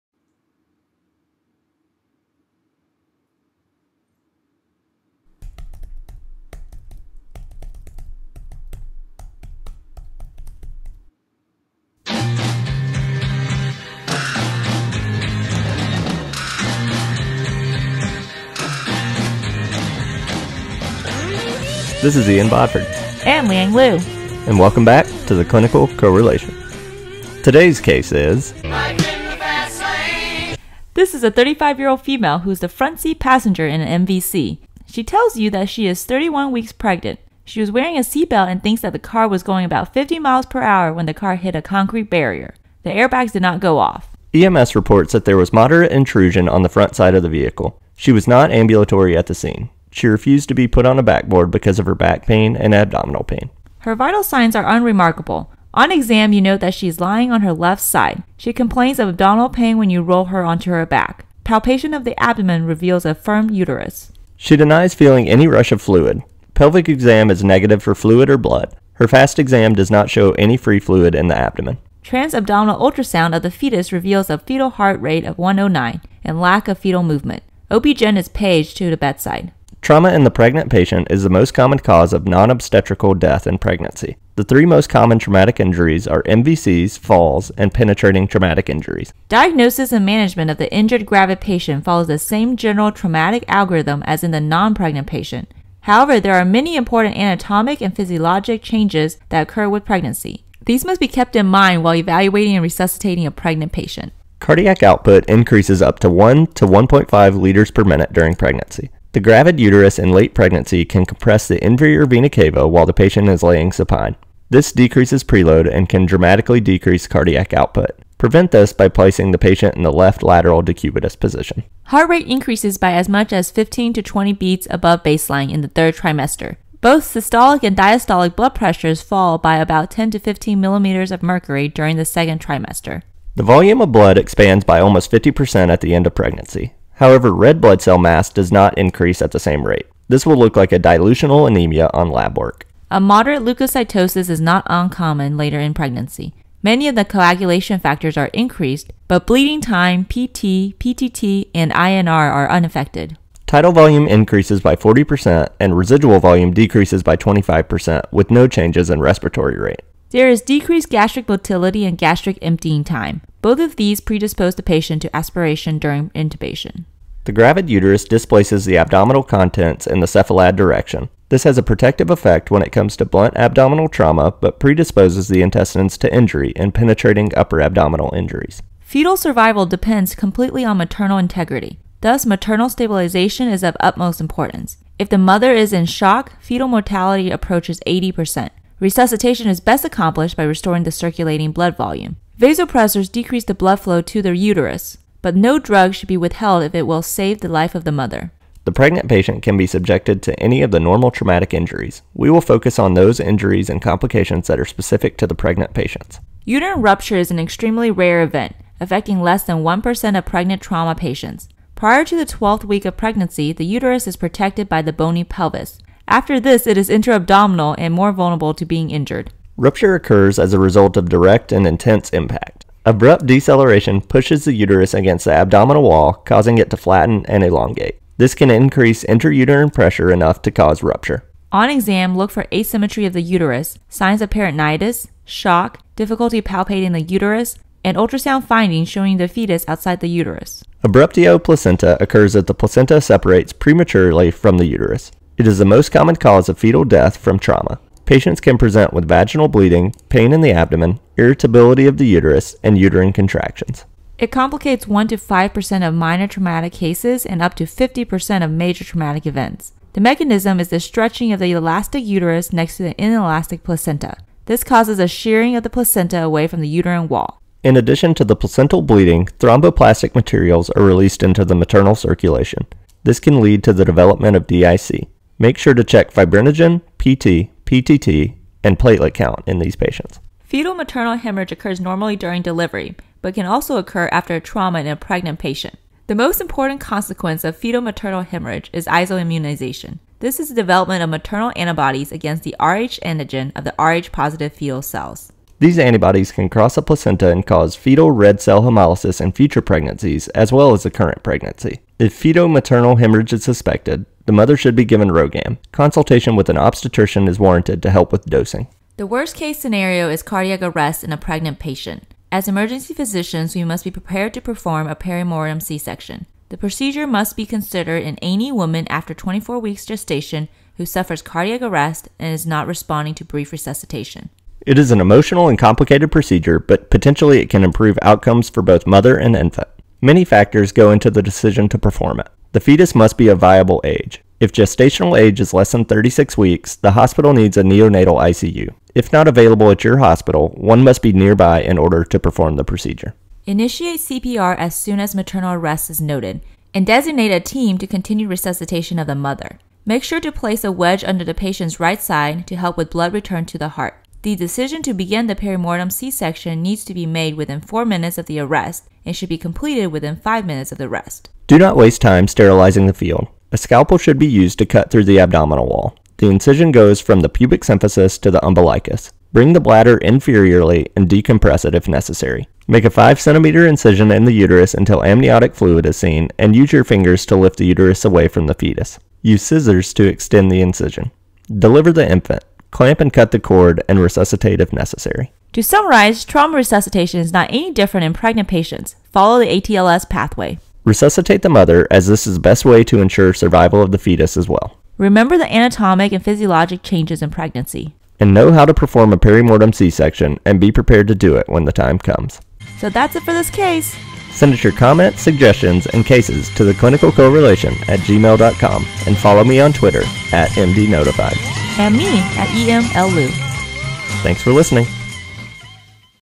CC 14: Life in the Fast Lane
CC 14 discusses briefly the physiologic changes in pregnancy and specifically focuses on the trauma resuscitation of a pregnant female. Are you ready to perform a perimortem c-section?